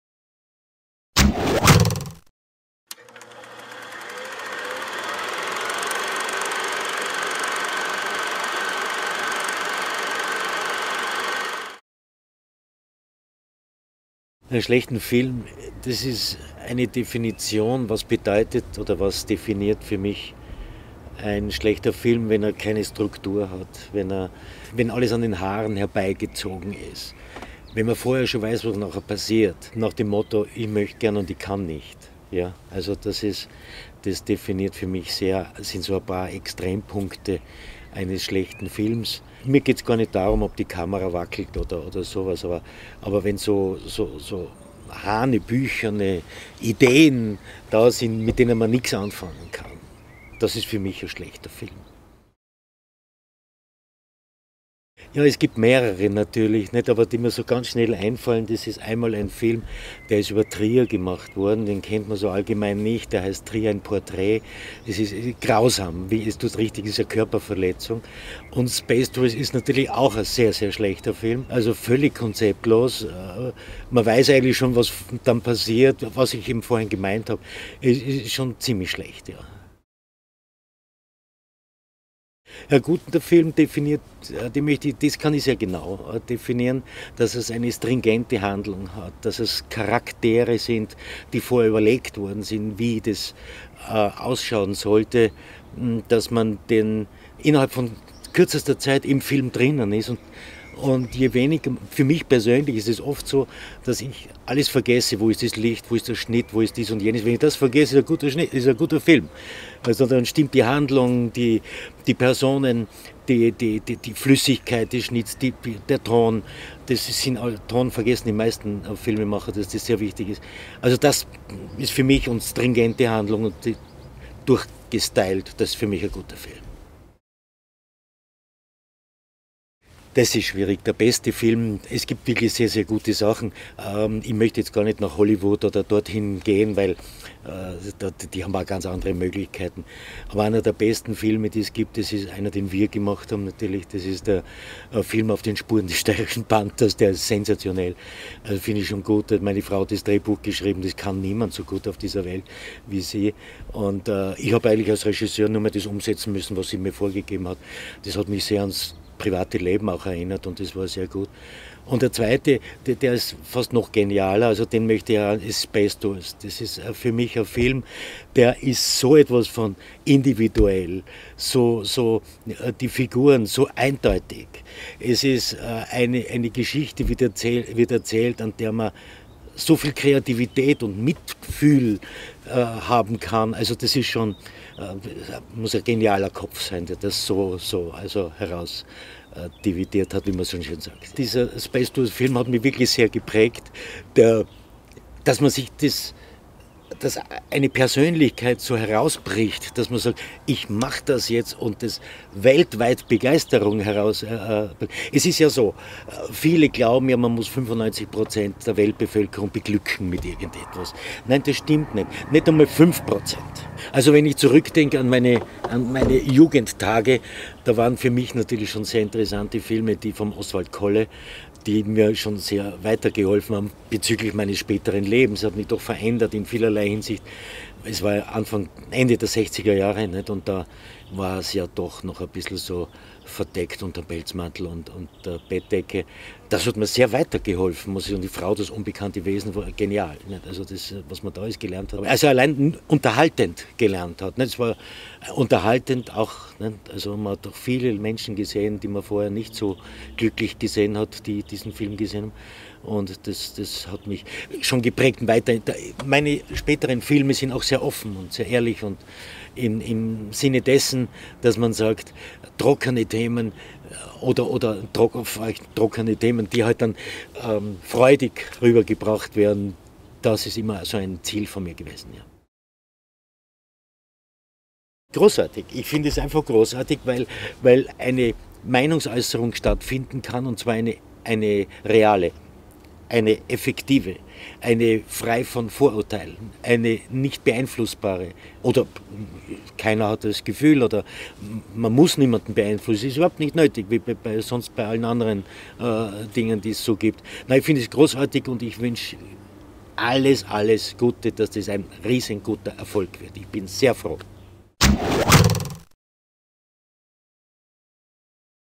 cuke.it INTERVIEW ROLAND BERGER
cuke.it Interview mit Filmemacher Roland Berger